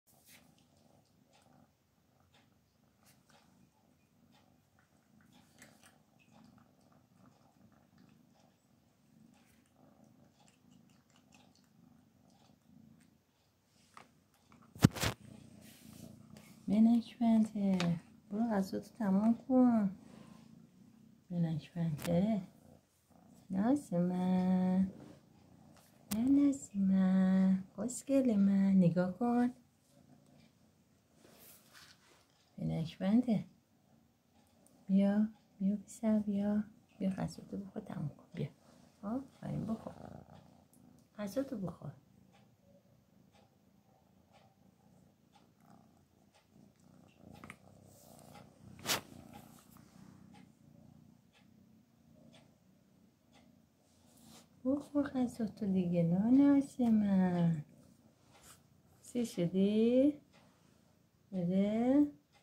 Black Panther's appetite has decreased, so we are trying to make her eat more food,
Black Panther's appetite has decreased, so we are trying to make her eat more food, because her sense of smell has decreased due to the cold.